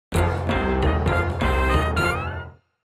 Cartoon Network - Generic Endtag Logo (1999-2016, Remastered in 4K)
Starting with Regular Show episode "The Key to the Universe", CN changed its endtag logo with a new one, replacing the well-known Cartoon Cartoon logo, which has been used since 1999.